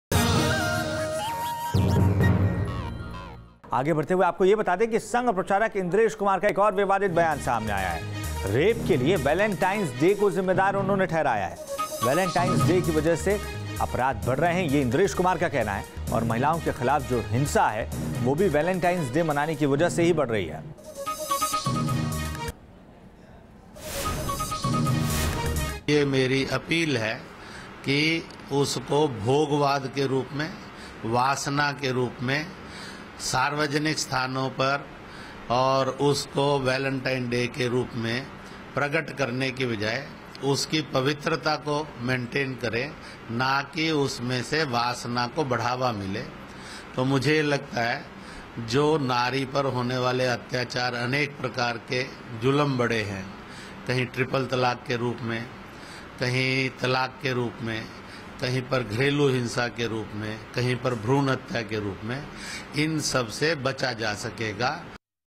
Valentine's Day responsible for rape, says RSS leader Indresh Kumar
Valentine's Day responsible for rape, illegitimate children and violence against women, says RSS leader Indresh Kumar